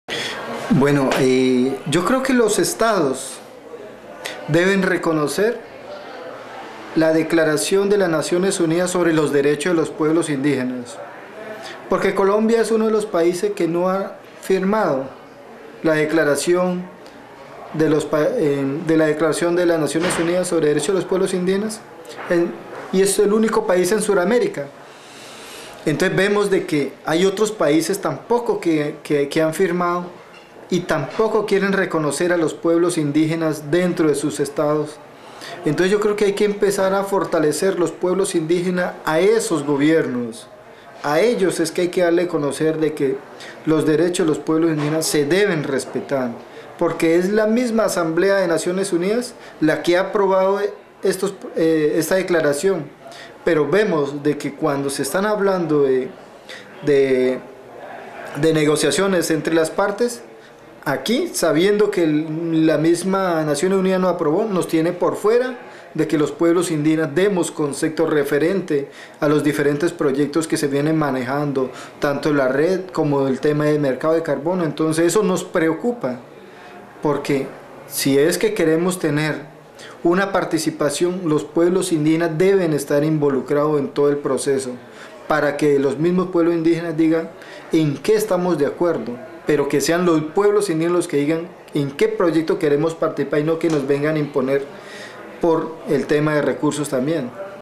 UNFCCC T13 Jorge Gabriel Furagaro Kuetgaje 5
Raw footage video interviews (work-in-progress info video documentary on REDD for Indigenous Peoples).
Posted for translation/transcription purposes over the long distance.